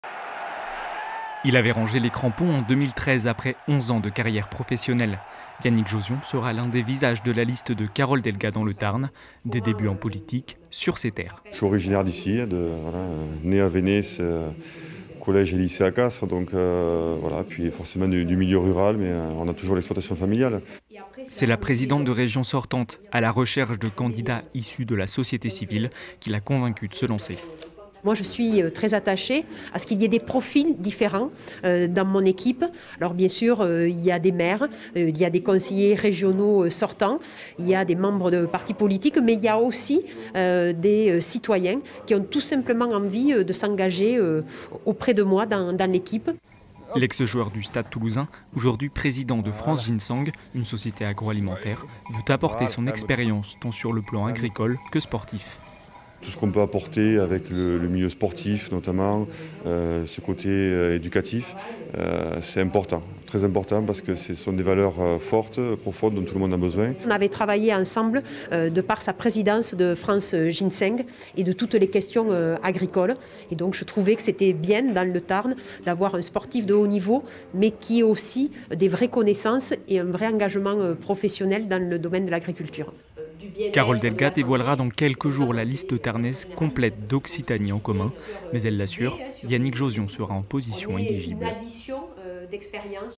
Elections Régionales : l'ex rugbyman Yannick Jauzion candidat dans le Tarn
Carole Delga avait annoncé la candidature d'une personnalité tarnaise sur sa liste Occitanie en commun. Il s'agit de Yannick Jauzion. L'ex rugbyman international et centre du Stade Toulousain se lance en politique, au côté de la présidente de Région sortante.